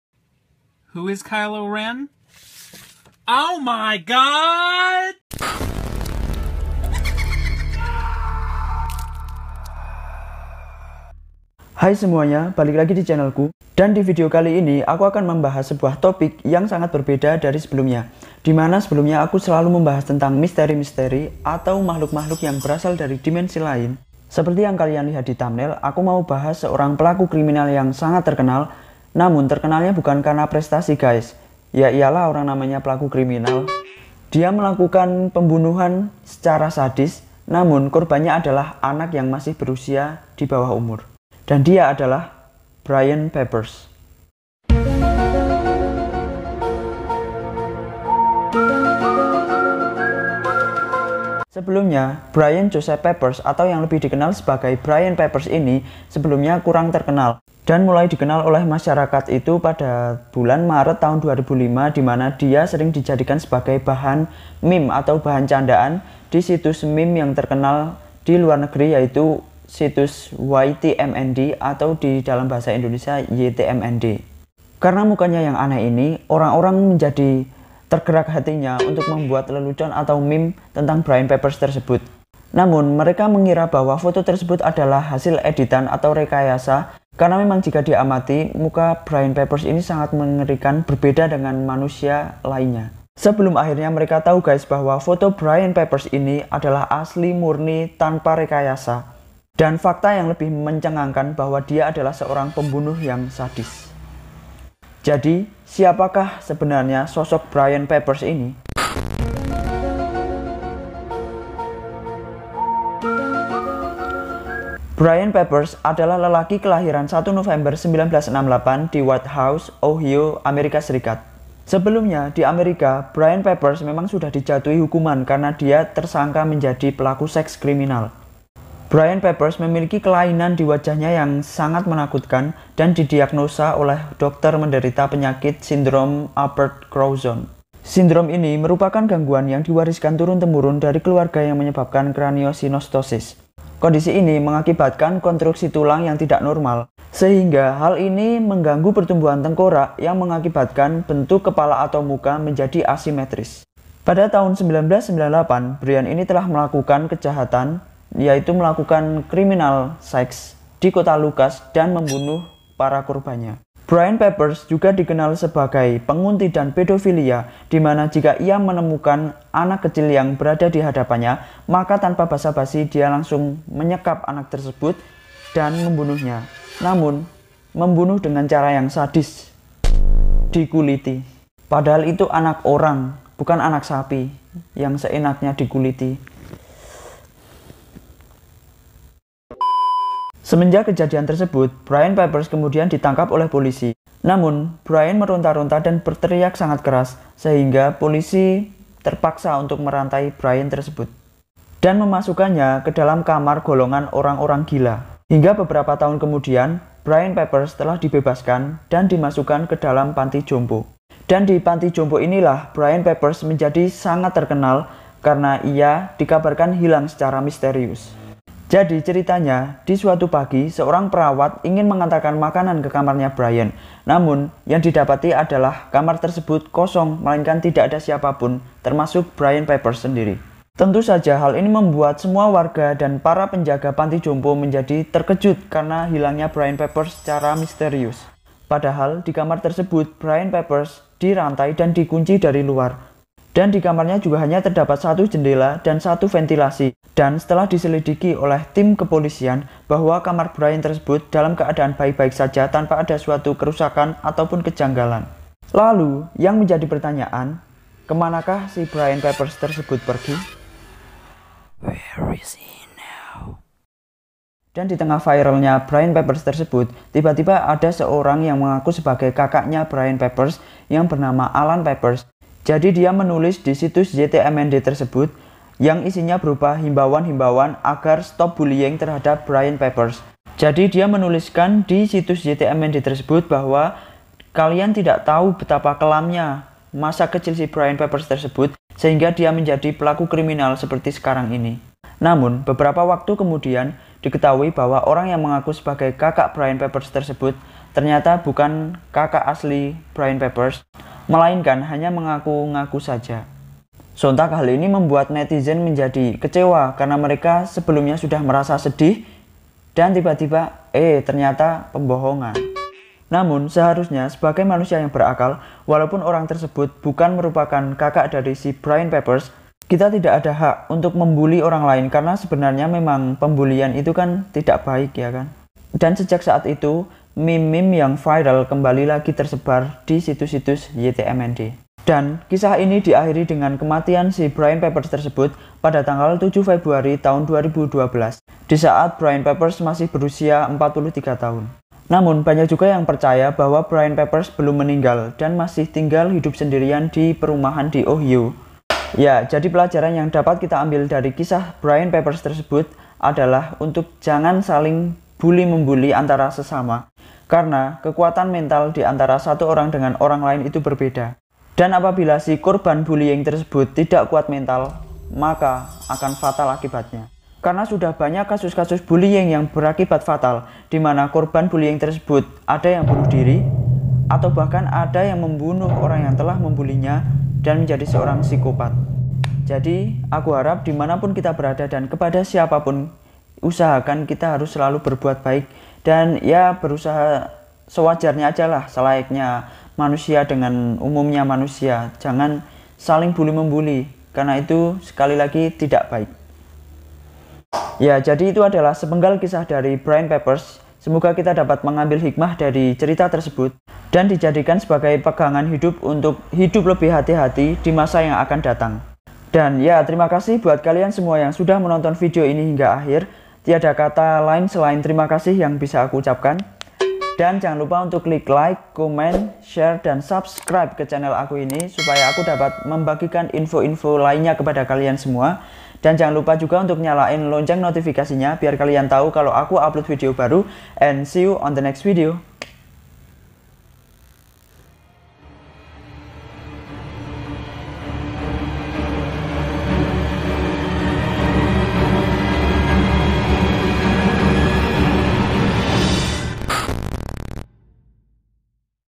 Brian Peppers | Siapakah dia??
Assalamualaikum semua, Sebelumnya Terimakasih buat yang sudah klik video ini, dan di video kali ini aku mau share tentang Brian Peppers, seorang Psikop4t asal Amerika yang menderita Syndrome Apert Crouzon. Penasaran ? Tonton sampai habis ya. Hehe